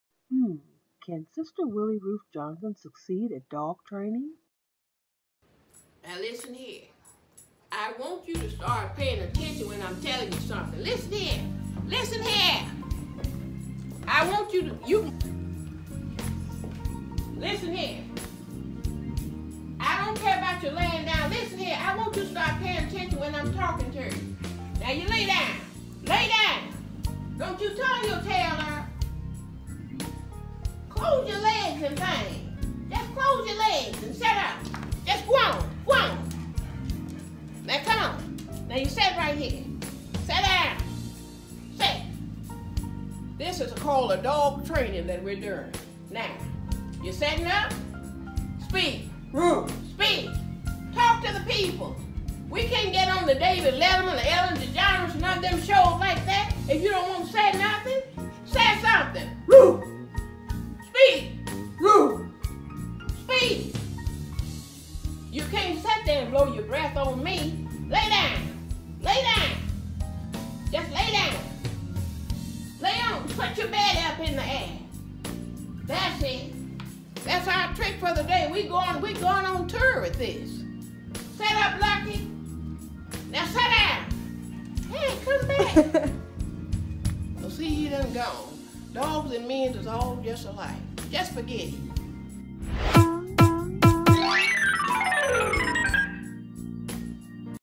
WRJ dogtrainingClass1 comedy
Comedienne Sistah Willie Ruth Johnson is an expert on dogs. She is technologically, illogically certifiably trained.